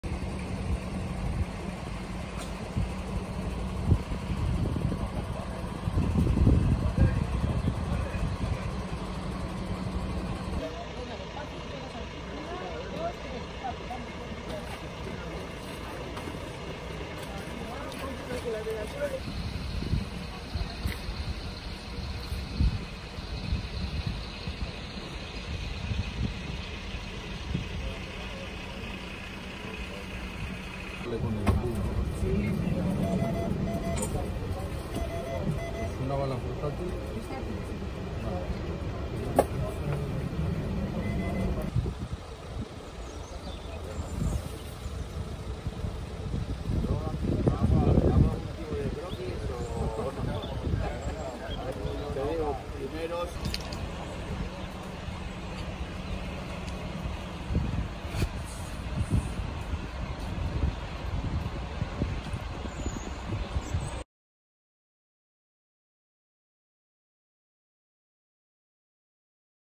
Accidente contra árbol en Usera
Un accidente de tráfico ha terminado con un vehículo ardiendo tras impactar contra un árbol en el Camino de Perales, 96, en Usera. Bomberos del Ayuntamiento de Madrid han extinguido las llamas del turismo en el que ya no había nadie en el interior. SAMUR-PC ha atendido a un varón de 36 años con un trauma facial, estaba agitado, ha sido intubado y trasladado en un colchón de vacío por mecanismo lesional. Ha sido transferido al Gregorio Marañón con preaviso. Policía Municipal realiza el atestado.